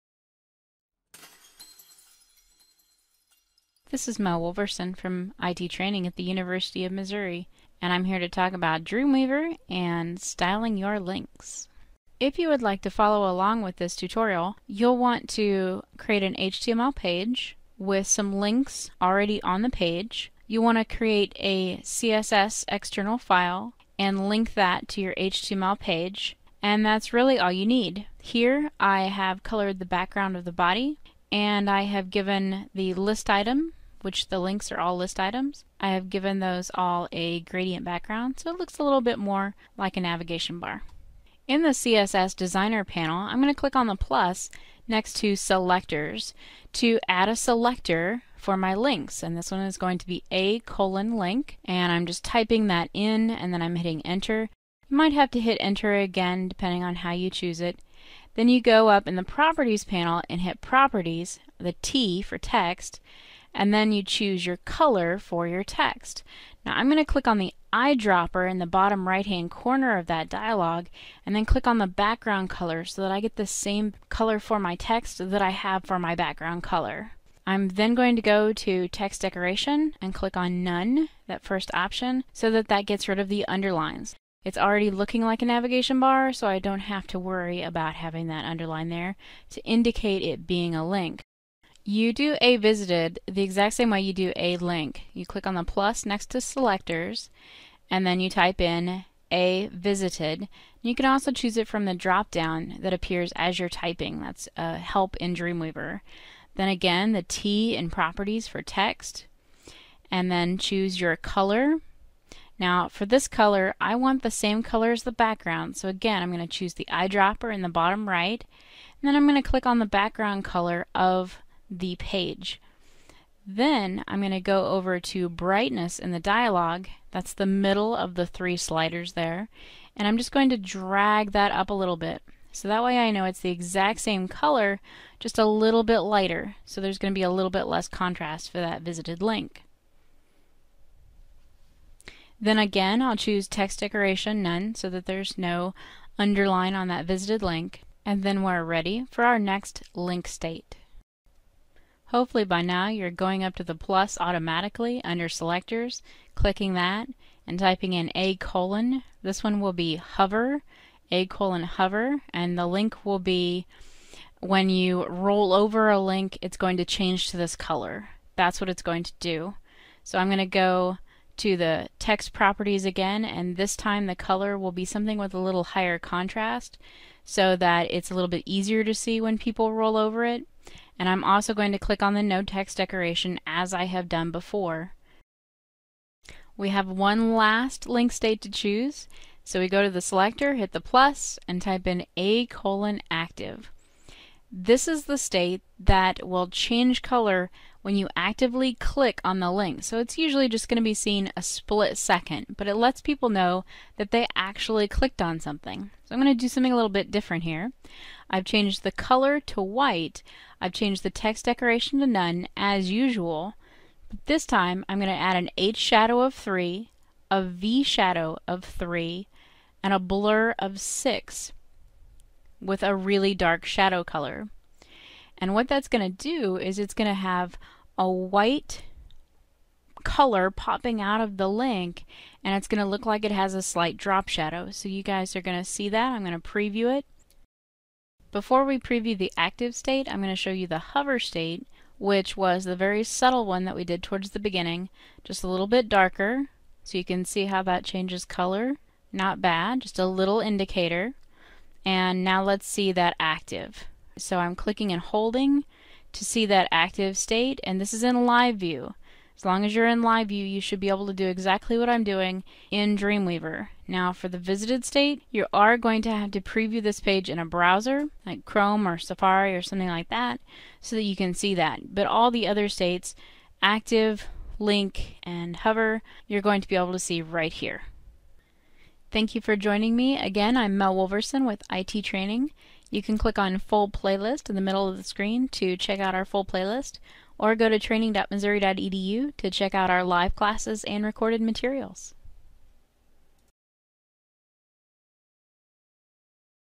Dreamweaver CC - Styling Links
Mel. Wolverson teaches you how to style your links using CSS in Dreamweaver CC. Video includes: default link color, hover link color, visited link color, and active link color.